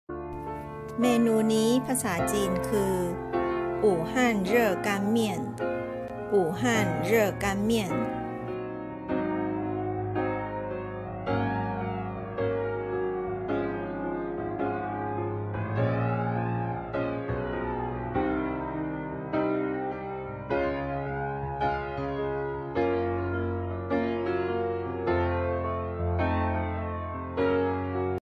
武汉热干面
มารู้จักเมนูอาหารต่างๆ เป็นภาษาจีนกันค่ะ

เรียนจีน หยางเหล่าซือ Study Chinese#Teacher Yang#武汉热干面#Chinese#Food#อาหาร#ภาษาจีน#เมนูภาษาจีน